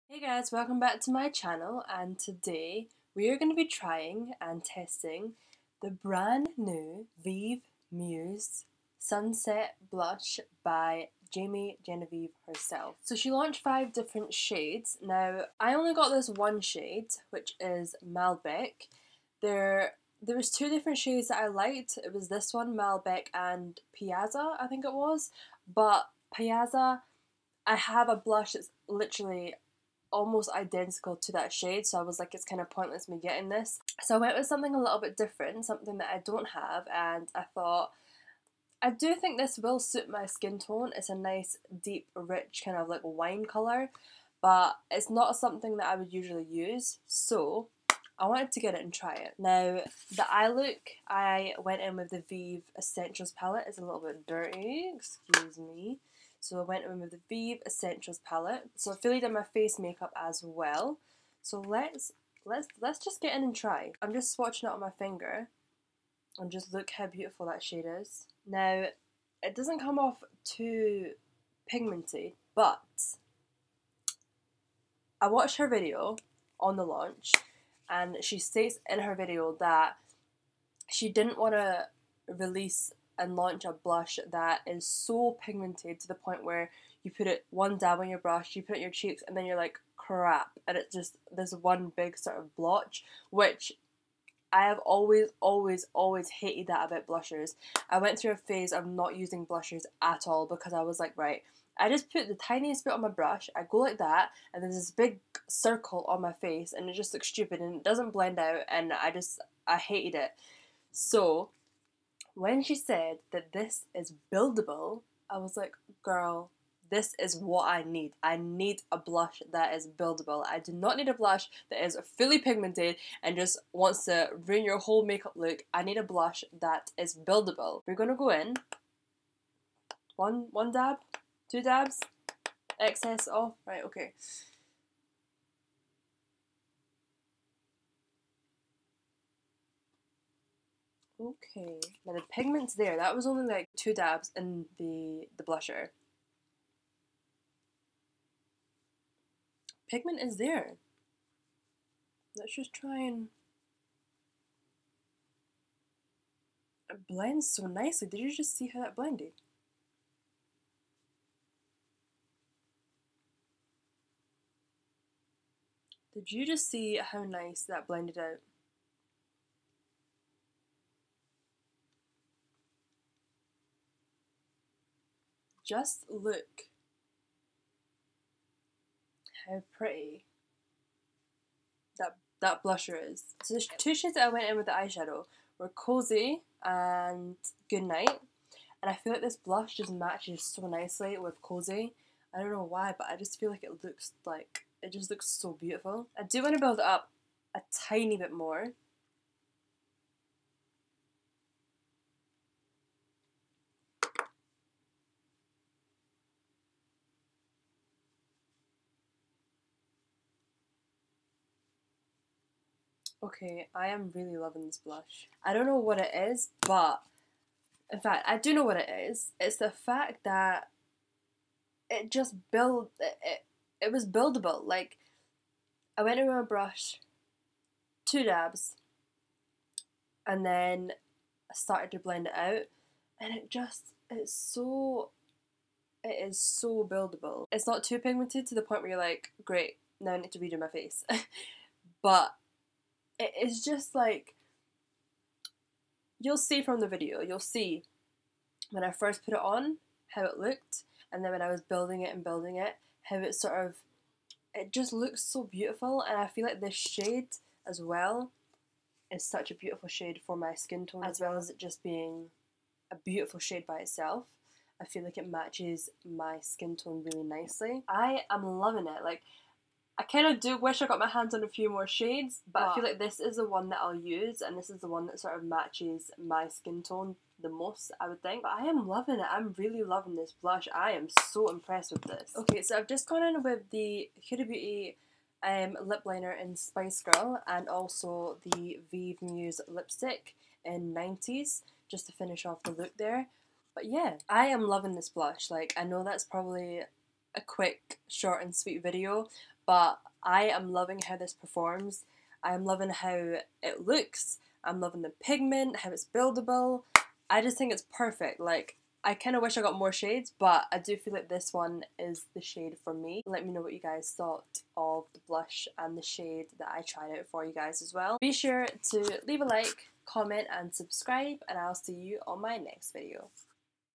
JAMIE GENEVIEVE SUNSET BLUSH TUTORIAL AND REVIEW | VIEVE MUSE
Tutorial and review on the brand new Sunset Blush by Jamie Genevieve, Vieve Muse

Sunset Blush - Malbec